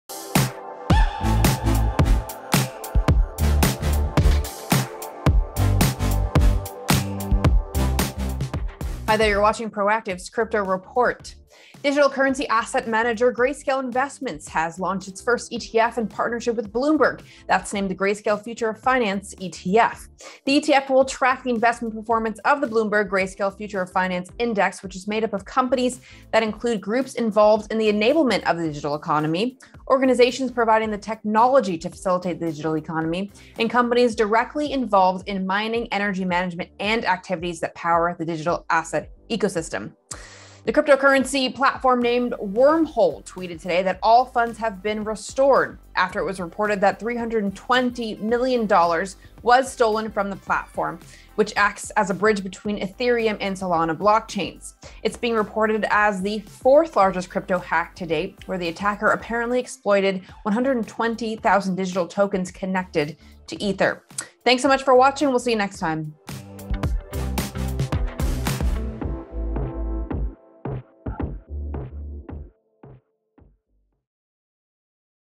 The Crypto Report: Grayscale launches its first ETF, Wormhole says $320M in stolen funds restored
The fund will track the investment performance of the Bloomberg Grayscale Future of Finance Index which is made up of companies that include: groups involved in the enablement of the digital economy, organizations providing the technology to facilitate the digital economy, and companies directly involved in mining, energy management, and activities that power the digital asset ecosystem

The cryptocurrency platform named Wormhole tweeted today that "all funds have been restored" after it was reported that over $320 million was stolen from the platform, which acts as a bridge between the Ethereum and Solana blockchains. It is being reported as the fourth-largest crypto hack to date, where the attacker apparently exploited 120,000 digital tokens connected to ether.